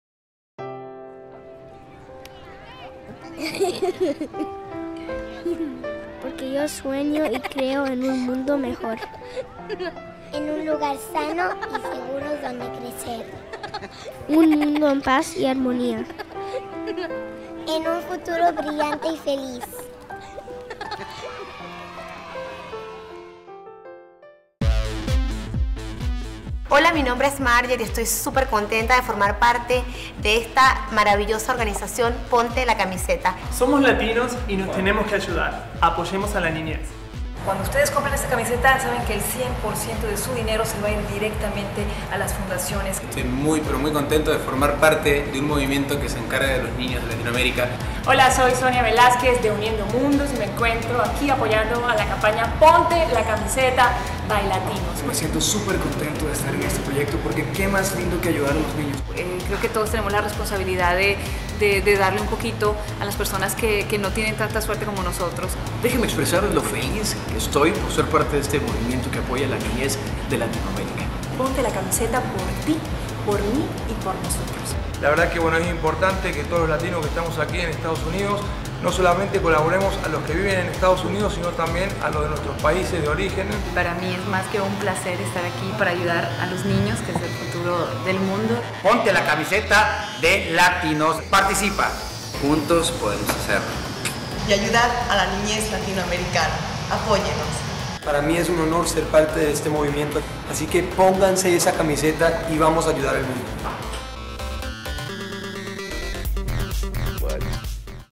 TESTIMONIALES PLC BY LATIN US
Testimoniales para la campaña "Ponte la Camiseta by Latin US"
Cliente: Latin US
Agencia: Innova